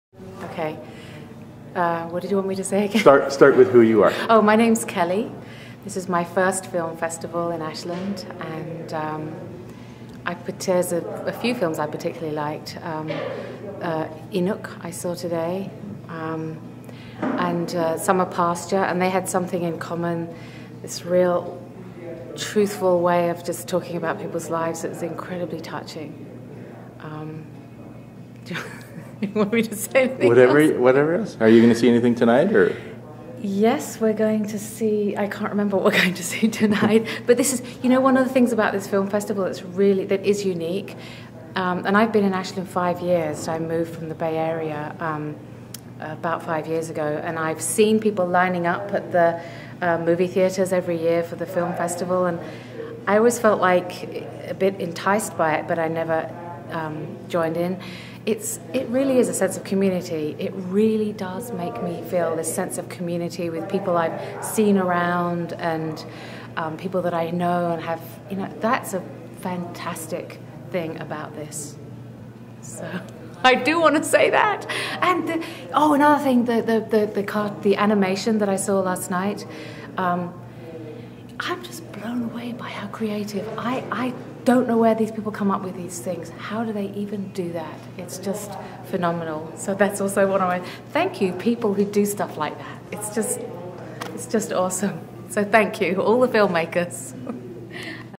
AIFF 2011 - Kelly.mov
Ashland Independent Film Festival, Ashland, Oregon 97520
AIFF is now in it's 10th year - Find out more at AshlandFilm.org
This is a podcast video from chatting with staff, fans, filmmakers and volunteers everyday during the festival.